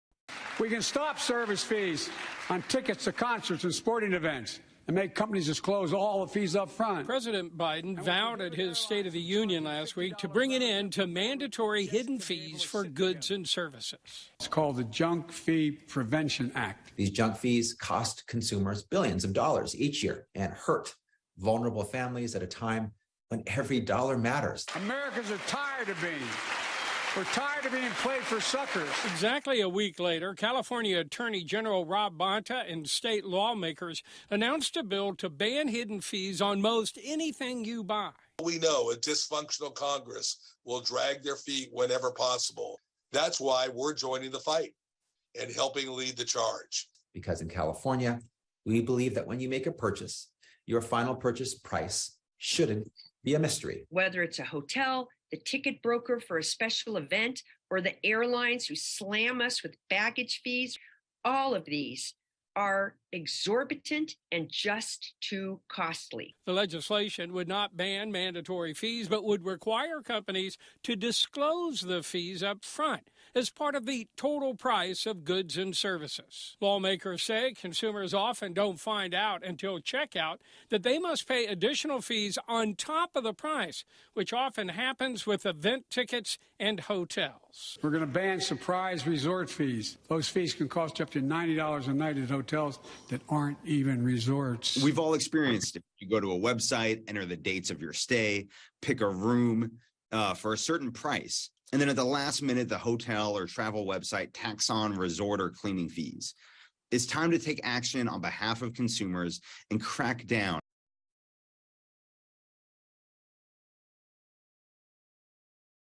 California lawmakers want to ban hidden fees on hotels, event tickets
President Joe Biden took aim at the State of the Union against hidden fees, but California lawmakers say they don't want to wait for congress to act.  "These junk fees cost consumers billions of dollars each year and hurt vulnerable families," said CA Attorney General Rob Bonta.